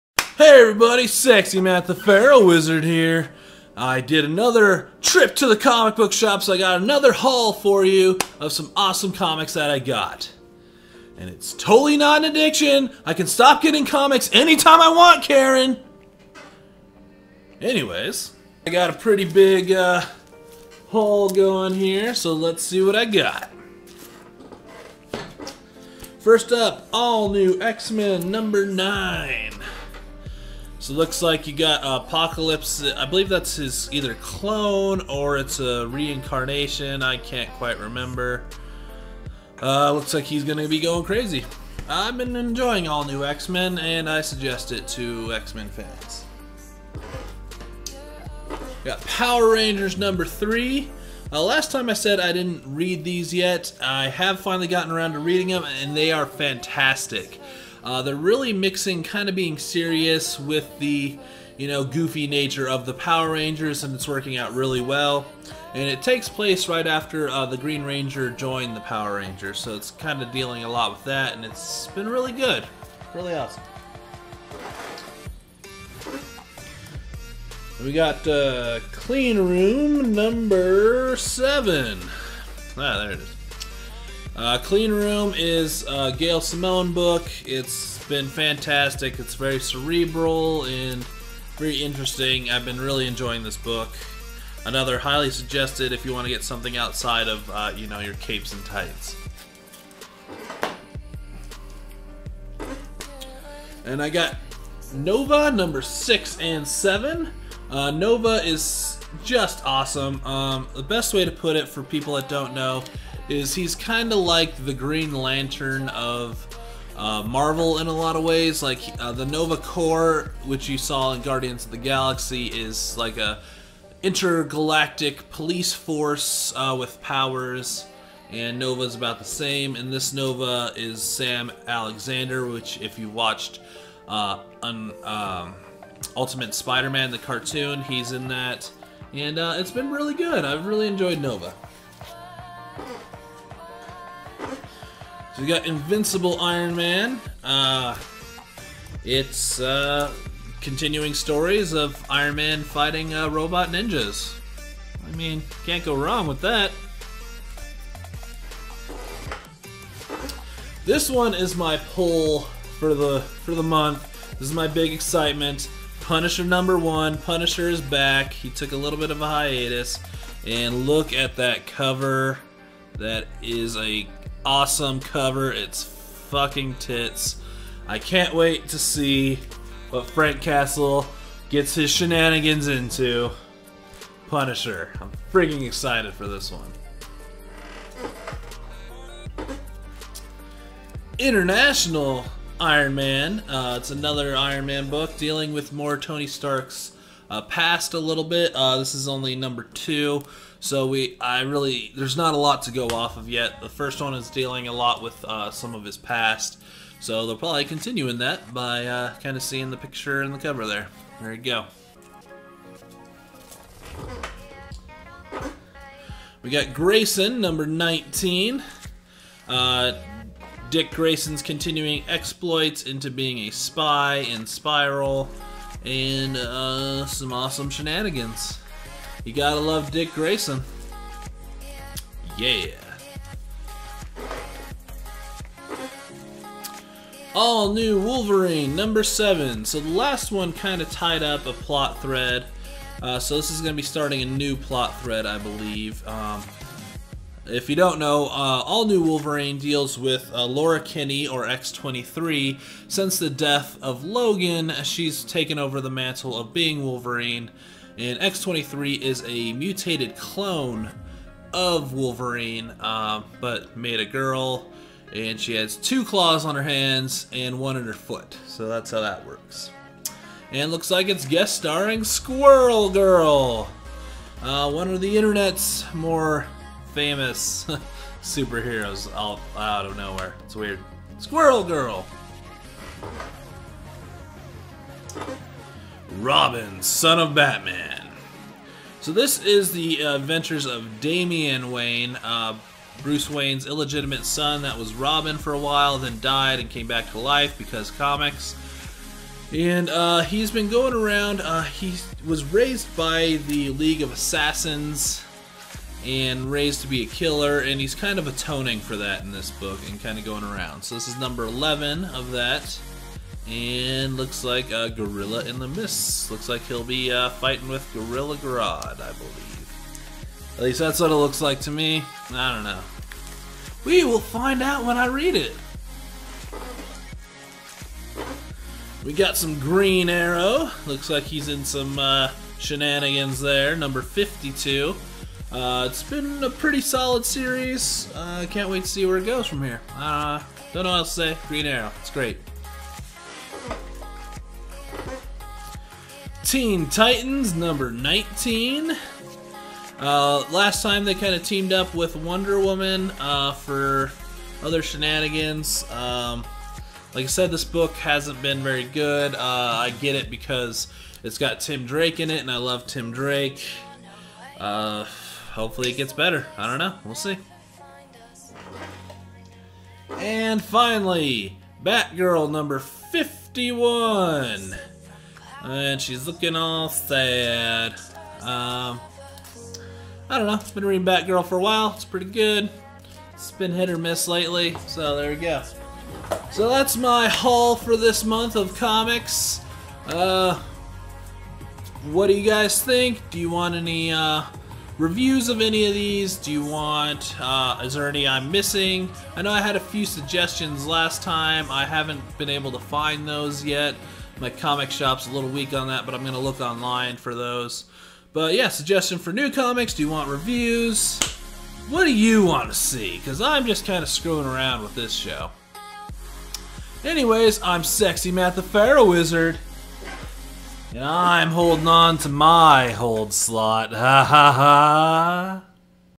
Comic Haul III: The Haul-ining
Sexy Matt came back from the comic shop. See what comics he is reading this month.